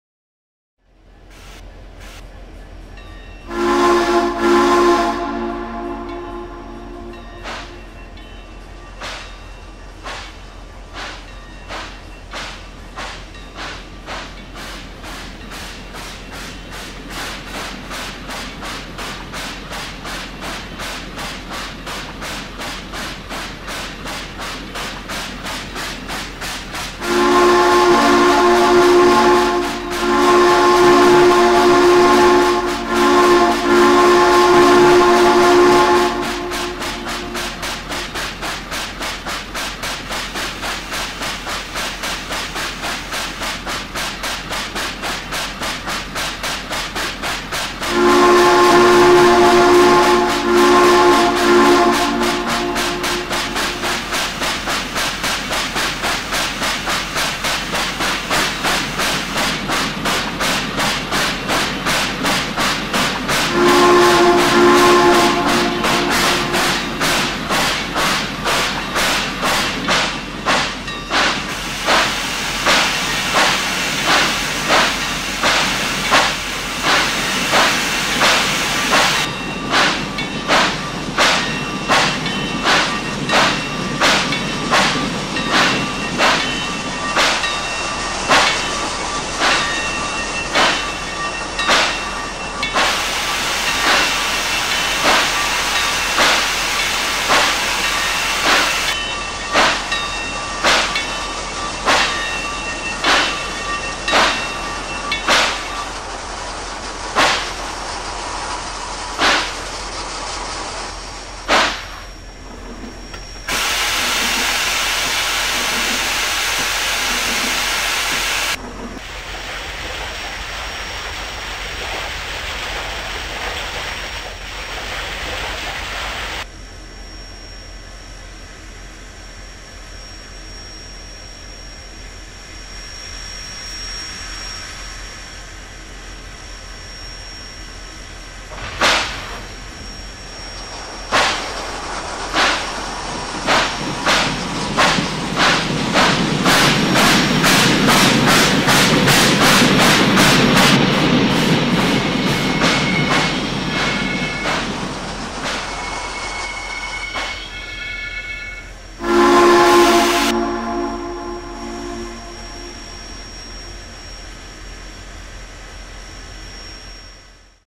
Frisco 1522's Epic Adventure Day 66 Part 175
Upon arriving at the 2nd Logging Camp for Spruce, the switch gets thrown and of course. Frisco 1522 and her Auxiliary Water Tender head up to the water tower near the small engine house. :)

This is just so that 1522's regular tender can have just enough water for of course the final miles up to the third and final summit as well as for the long descent on down towards the destination for Day 66: Webster Springs, West Virginia. ;)